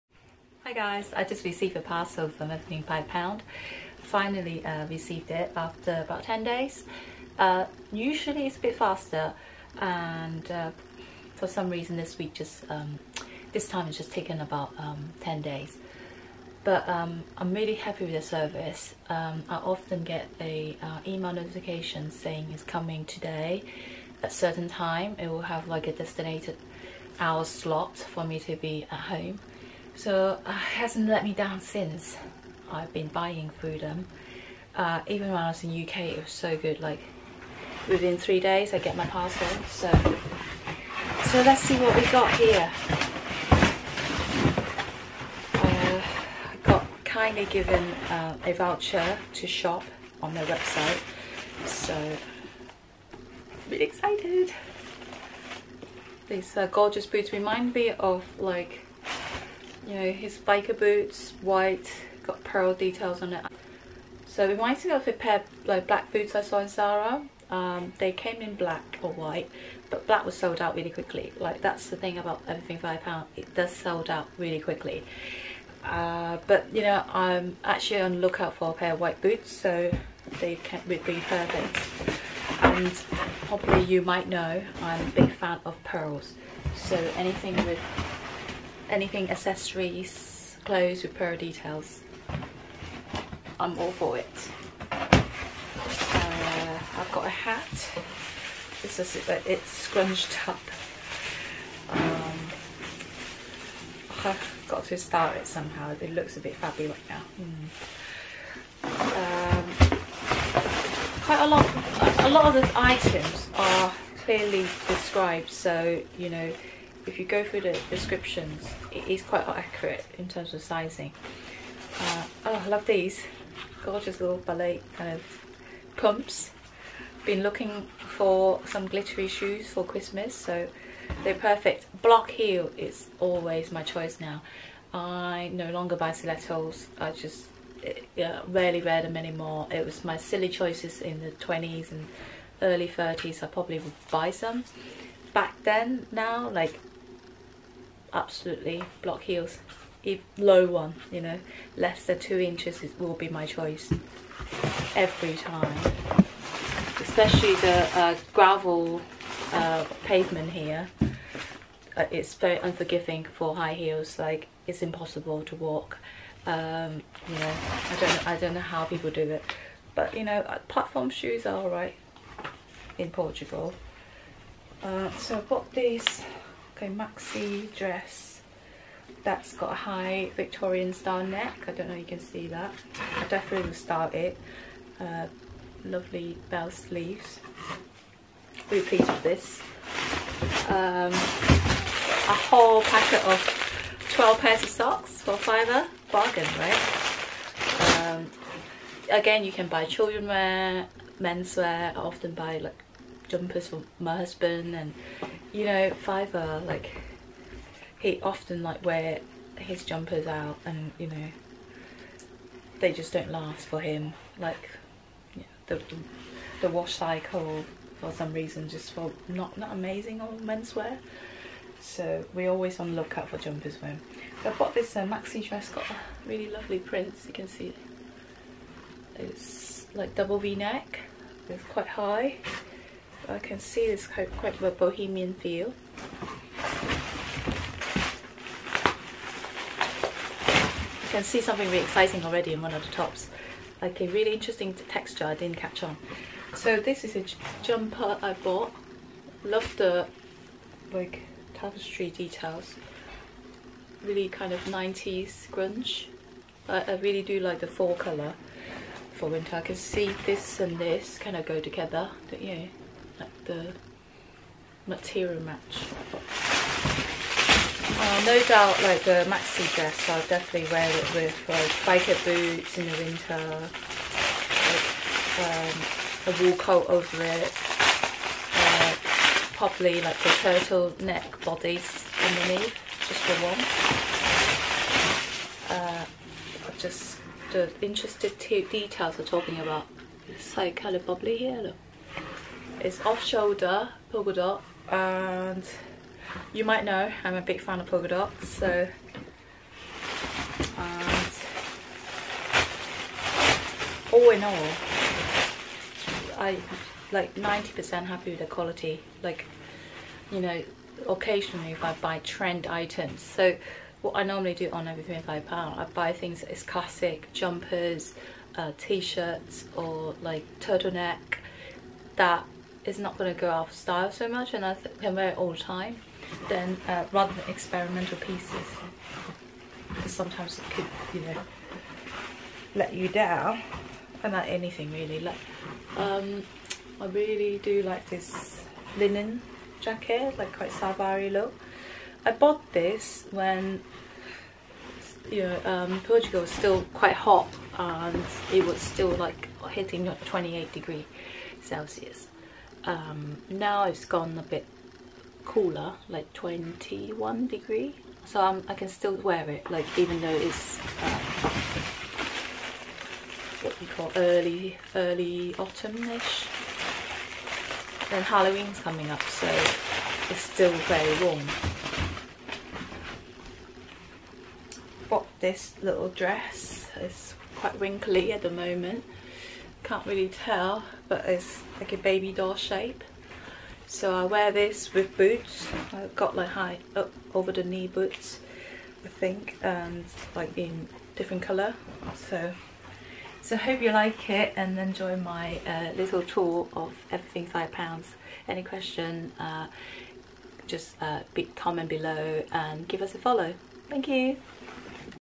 Everything5pounds unboxing (gifts) autumn/winter 2019 shoes, boots and transitional haul review
Here I am unboxing the gifts I received from Everything5pounds.com! I’ve been a massive fan for them for years, and I was kindly offered to feature their products. Just a little tip, if you want the best selection to shop, look at their latest arrivals, and they drop their products at 9 am every morning! Secrets is out, happy shopping!
Everything5pounds.com helps the collections from the high street to go into landfill.

Everything5pounds 2019秋冬取消裝箱-鞋子，靴子和過渡靴（禮物）
在這裡，我要把我從Everything5pounds.com收到的禮物拆箱！ 多年來，我一直是他們的忠實擁護者，並向我推薦他們的產品。 只需提示一下，如果您想購買最好的商品，請查看他們的最新到貨，他們會在每天早上9點退貨！ 秘密已經出來，購物愉快！
 Everything5pounds.com幫助大街上的藏品進入垃圾填埋場。